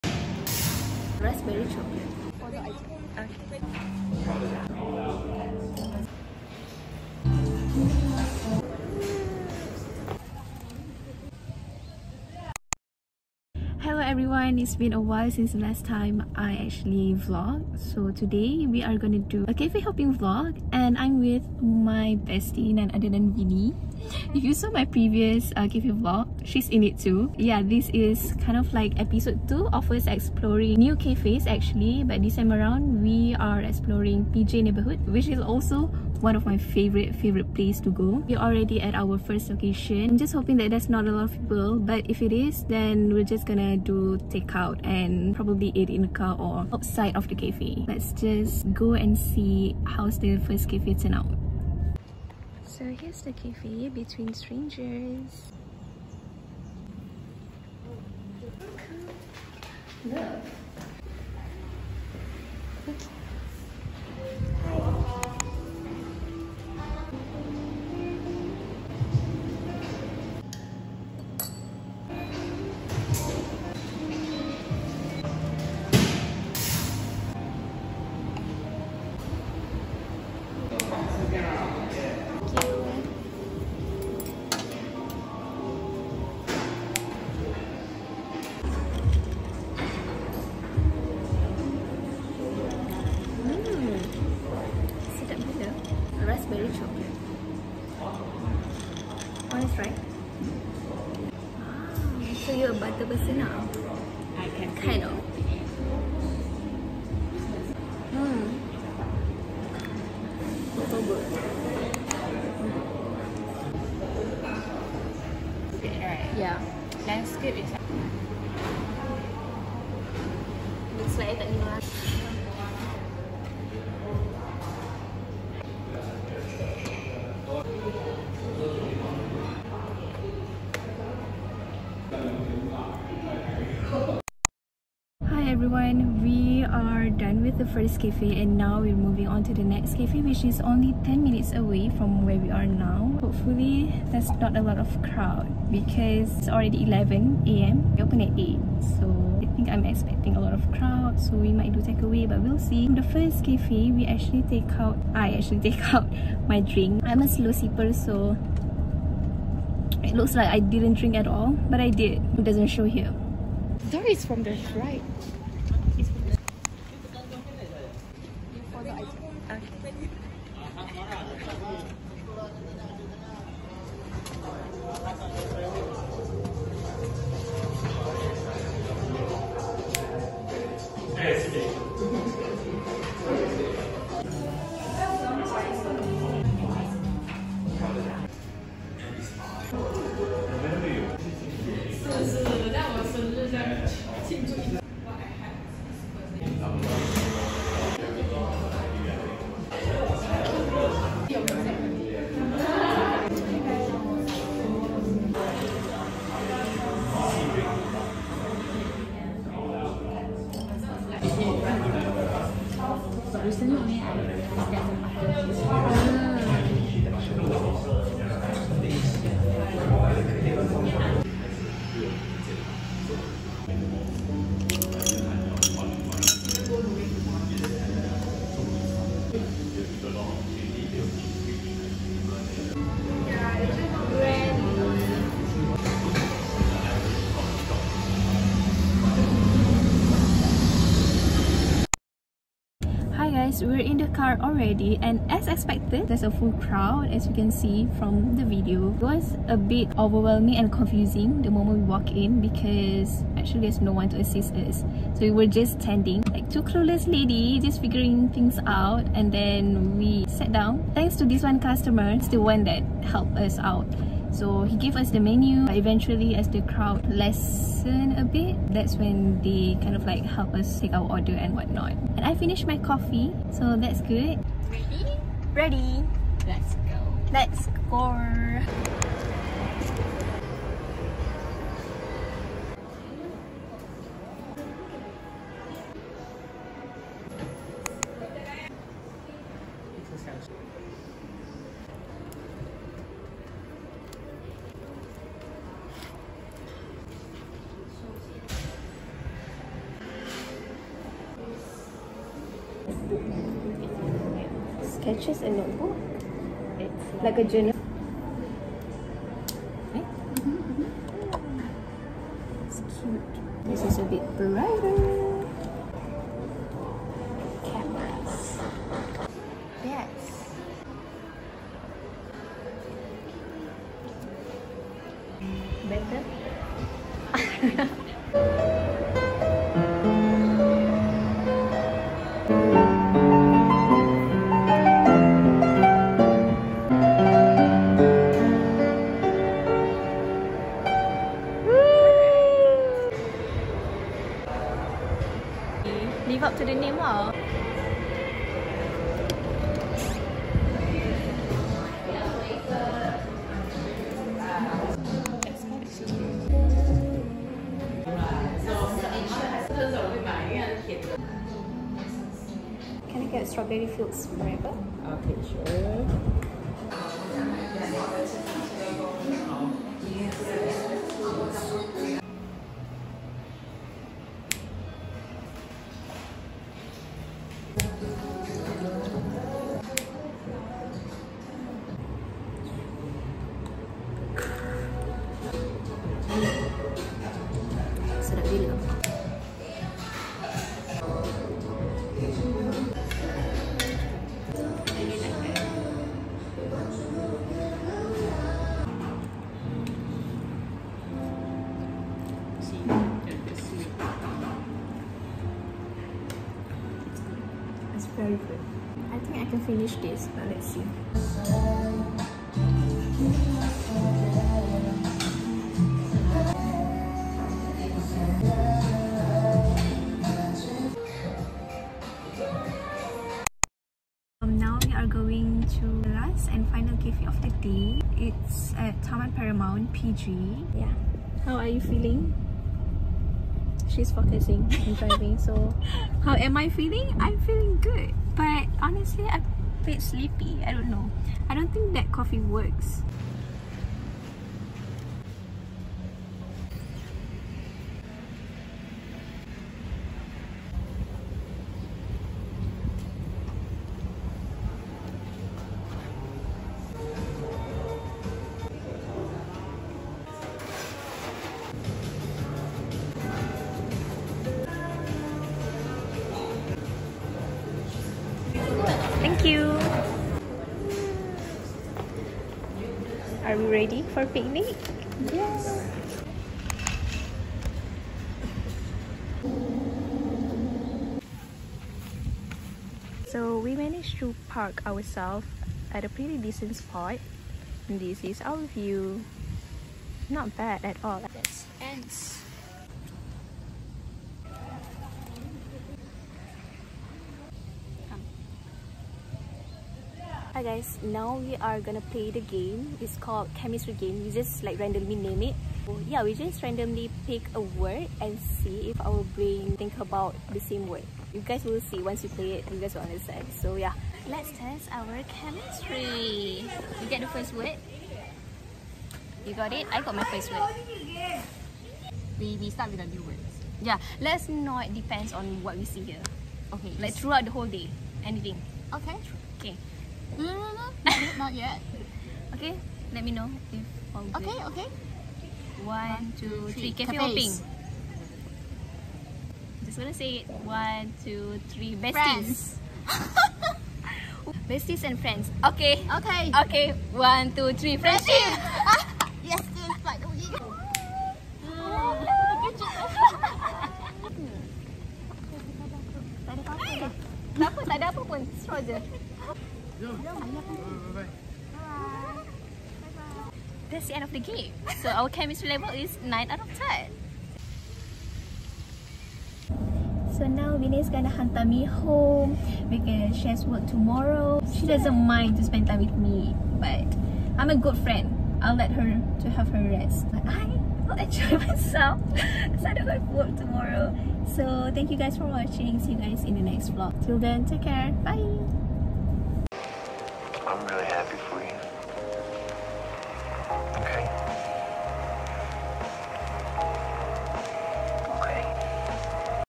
kl cafe hunting : exploring ‘minimal aesthetic’ cafes in pj ☕️
im a sucker for anything minimal and clean, and that goes to cafe choices as well! so in this cafe vlog- im bringing u guys along to my fav neighbourhood, pj (petaling jaya) as me and my bestie explore newly open cafes that share similar aesthetics. yes. loved that.

++ here’s the aesthetics pj cafes we went to :

☕️ Between Strangers
☕️ One Half
☕️ Lazy Sunday Club
☕️ By btterswt